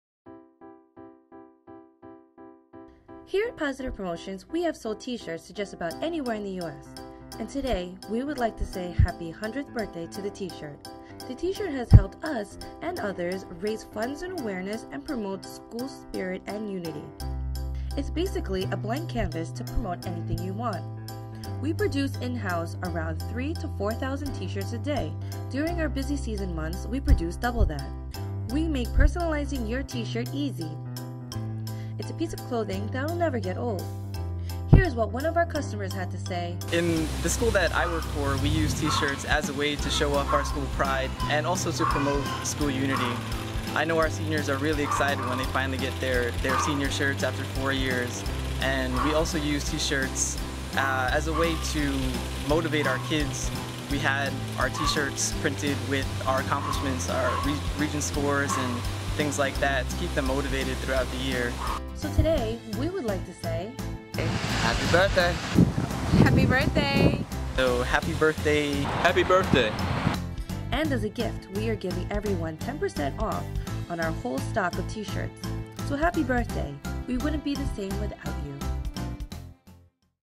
Happy Birthday T-Shirt!
Here at Positive Promotions we have sold t-shirts to just about anywhere in the US.  Today we would like to say Happy Birthday to the 100 year old T-shirt. The t-shirt has helped us and others raise funds and awareness, promote school spirit and unity and recognition.  It's basically a blank canvas to promote anything you want! ACT NOW OFFER ENDS 9/11/13!